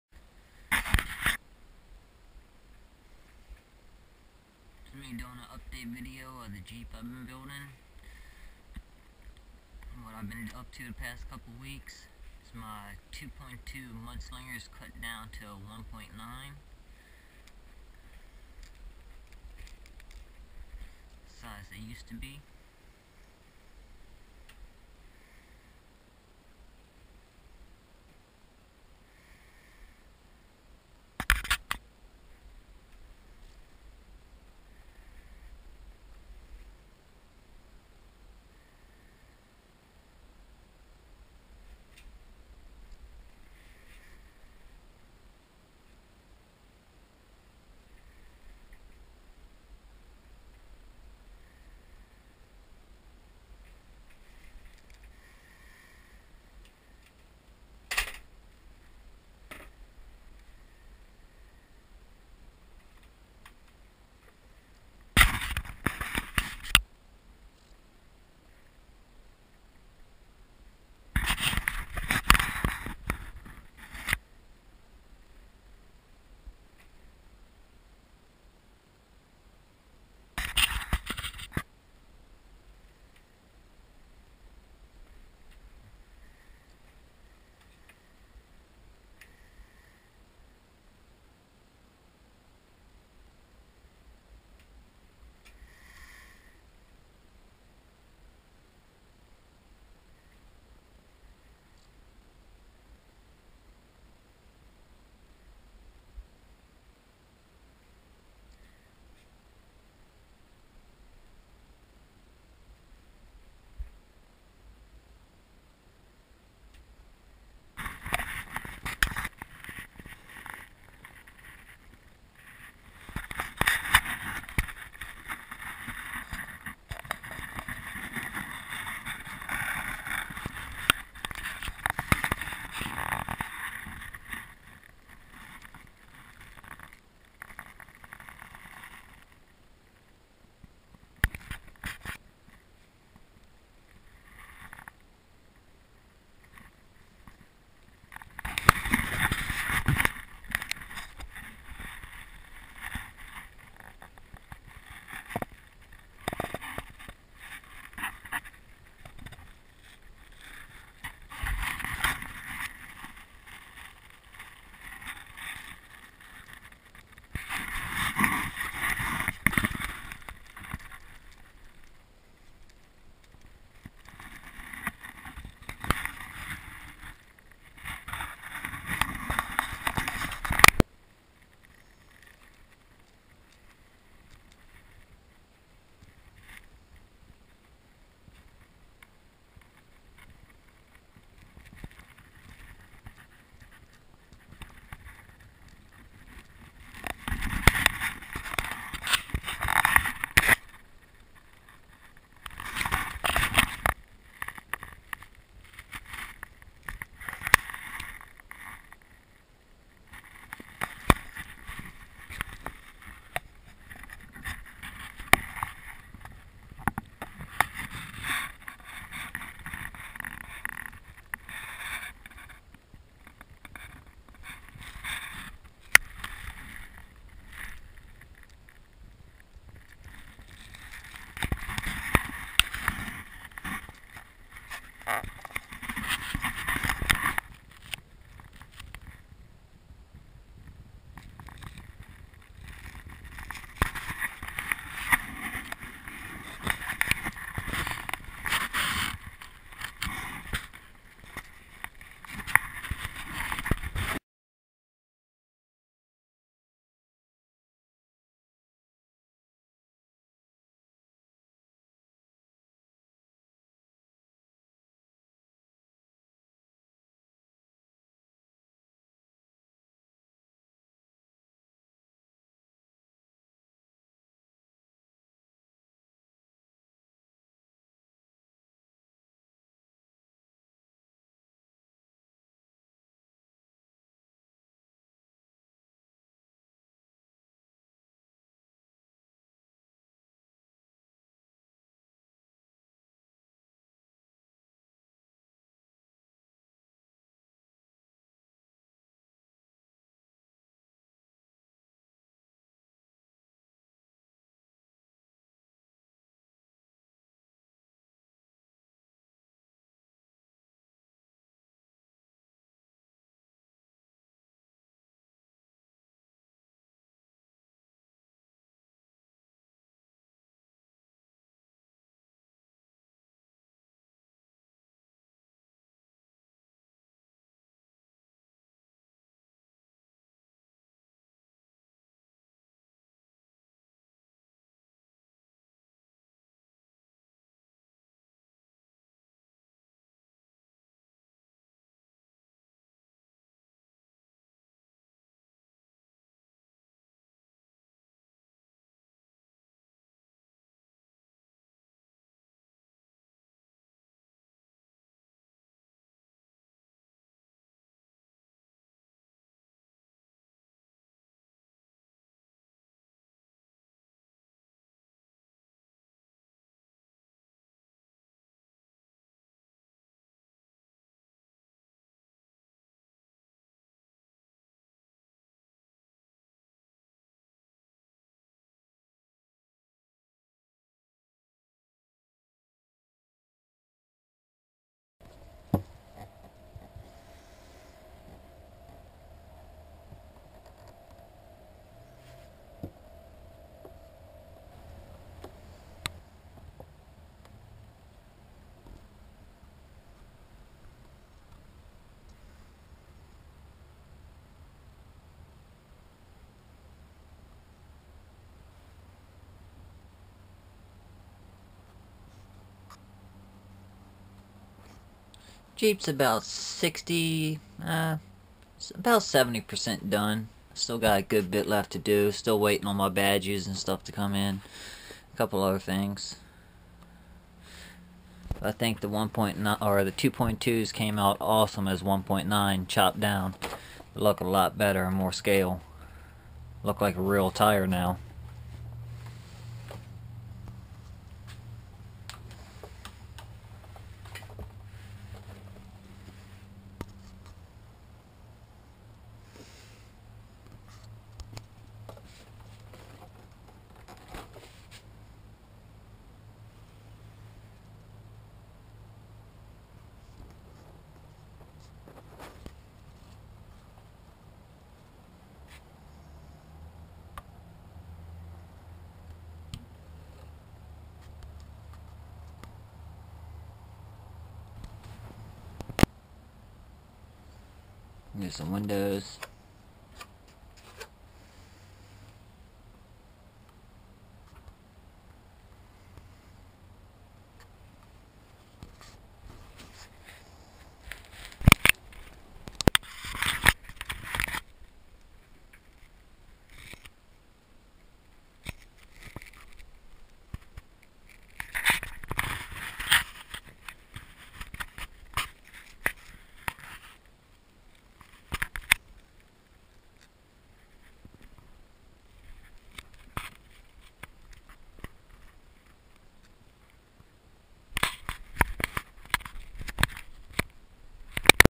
Update Jeep Wrangler JK 4door 4X4 Build Axial SCX10 RC4WD 2.2 mud slingers cut converted to 1.9.wmv
this is an update video/slide show of what ive been up to my latest build axial scx10 Jeep wrangler 4door 4x4 with full led light kit the interior is completly wrapped in twill weave carbon fiber i also took my RC4WD 2.2 Mud slingers and cut them down to 1.9 size and they look so much better than before they actually look like the real 1/1 scale tires do the  truck still has a bunch of work b4 im done but as soon as its finished be looking for a true to scale run video to be coming out thanks for watching please subscribe

heres a link on how to convert 2.2 to 1.9 from a buddy rcbitz